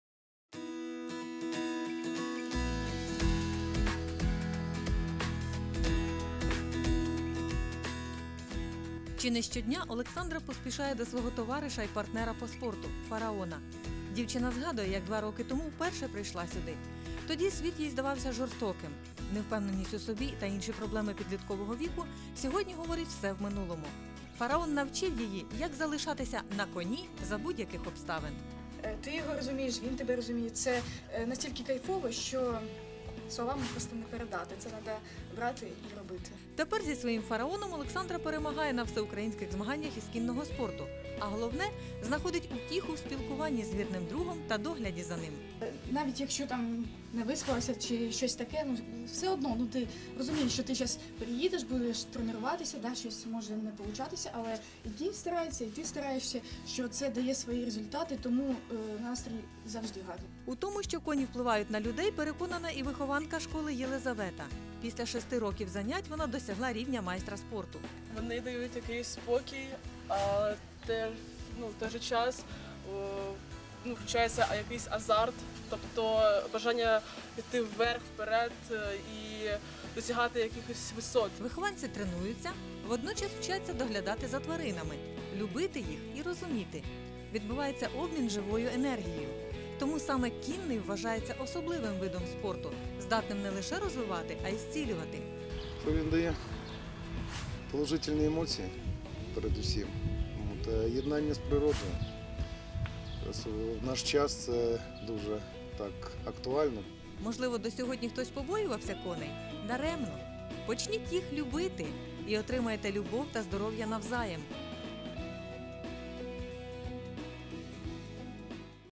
Іпотерапія - альтернативна терапія
Іпотерапія – це лікування за допомогою верхової їзди на коні під наглядом лікаря іпотерапевта або ж спеціально навченого інструктора верхової їзди. Унікальність такої терапії полягає в одночасному фізичному, психологічному та емоційно позитивному впливах на пацієнтів із неврологічними, психічними, фізичними та іншими серйозними порушеннями.